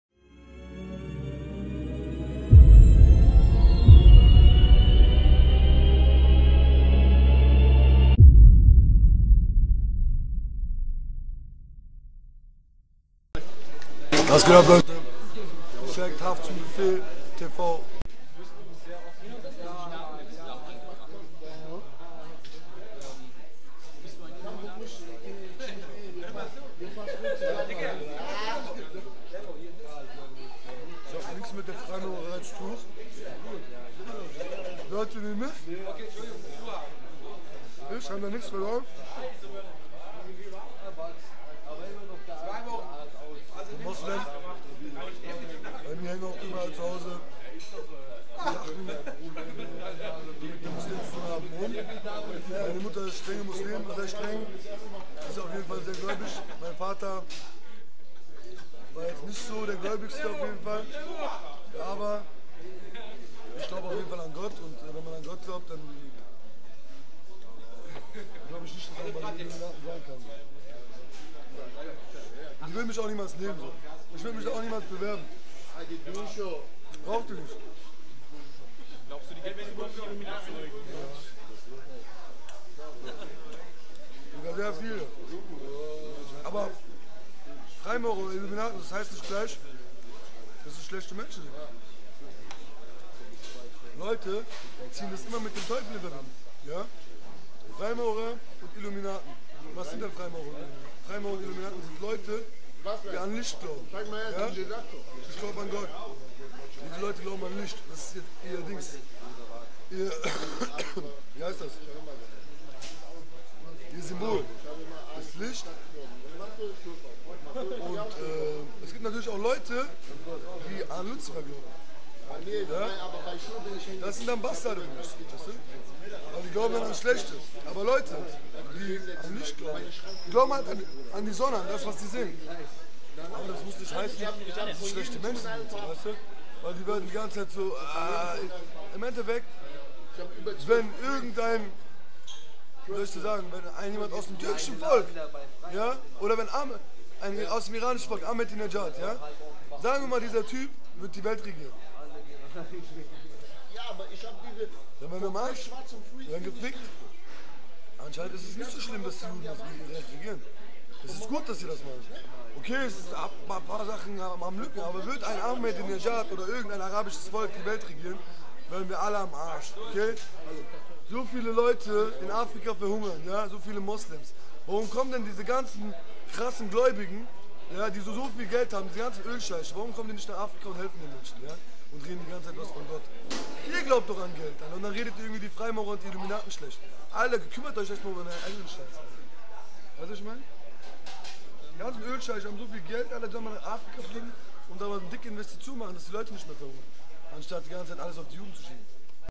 Haftbefehl über die Illuminatis! [Aus dem Haftzumbefehltv Interview]
Haftzumbefehltv war mit Haft und seinen Jungs nach dem Konzert in der Batschkapp bei KFC. Wir haben ihn u.a eine Frage zu den Illuminaten gestellt, da er immer mit den Illuminaten in Zusammenhang gebracht wird.